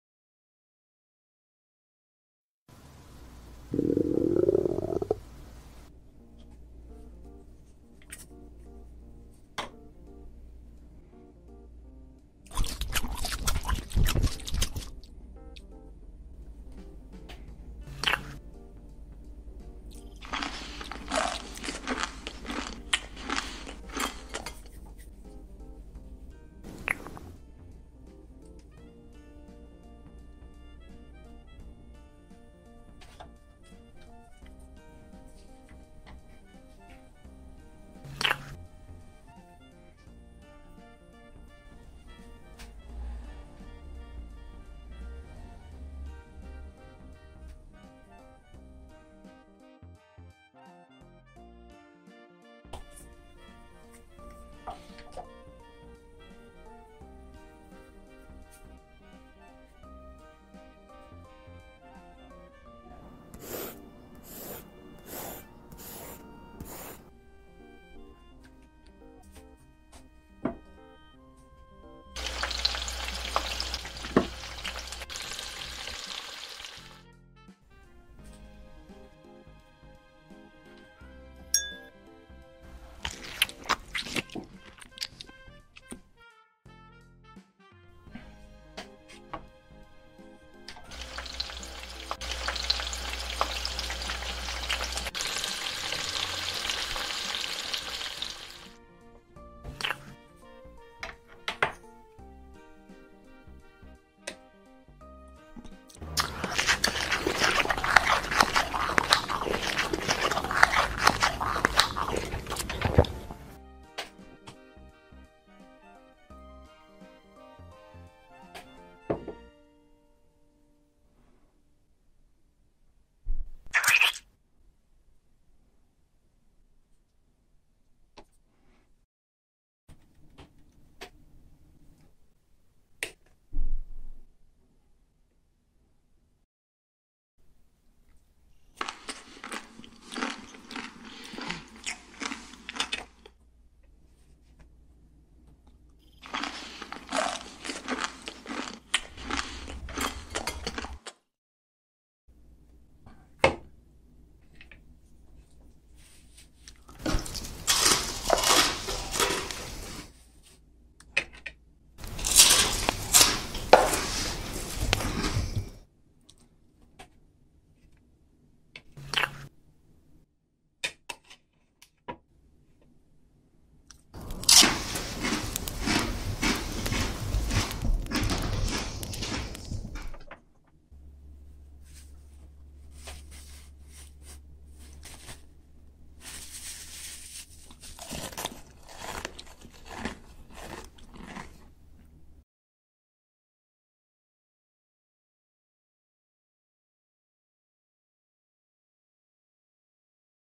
EATING STEAK COLOR CLAY ! Kluna Tik Dinner & Eating show ASMR eating sounds no talk 먹방 MUKBANG
ASMR eating sounds no talk! MUKBANG 먹방